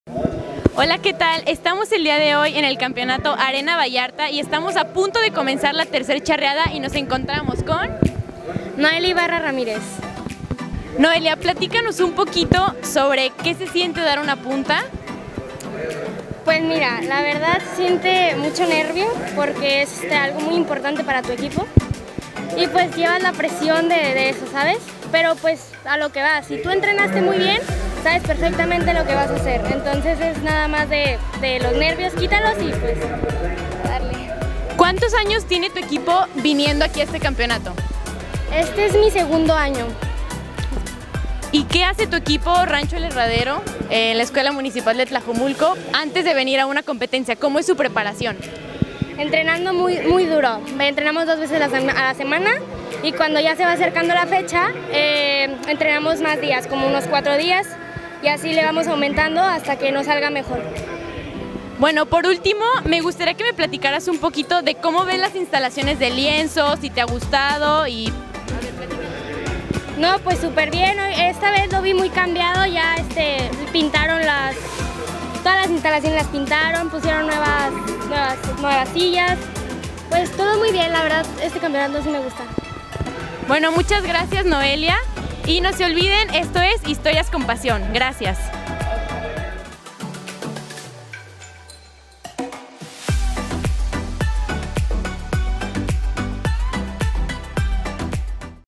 Lucero Rodríguez y Noelia Ibarra desde PuertoVallarta 2020
Hoy nos acompaña Lucero Rodríguez y Noelia Ibarra desde PuertoVallarta, Noelia Ibarra nos platica como se prepara para presentar punta.